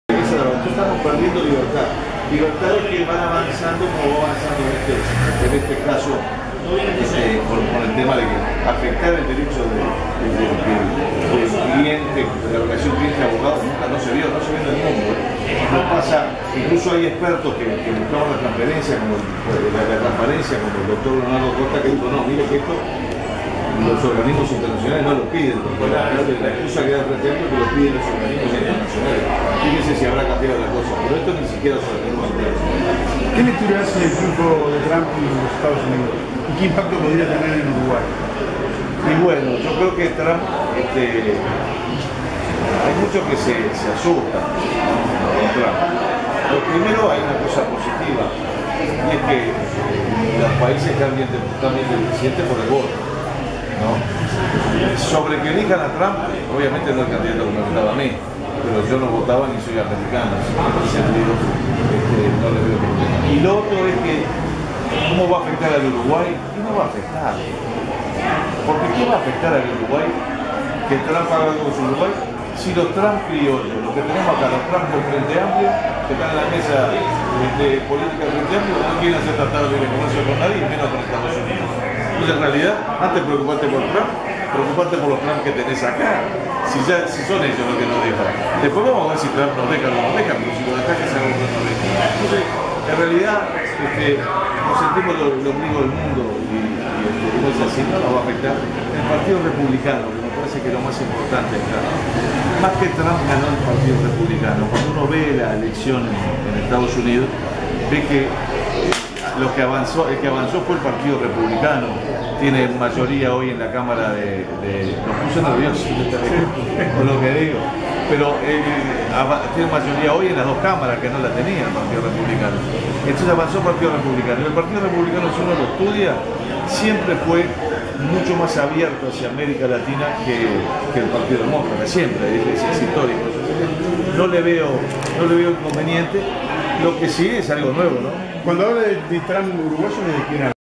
Pedro Bordaberry Recorriendo Colonia 2016 - 3
Una amplia recorrida realizó en este día viernes Pedro Bordaberry por ciudades del departamento de Colonia acompañado por legisladores de su sector conectándose a los efectos de cambiar ideas con dirigentes y ciudadanos de dichas ciudades.-    18-11-16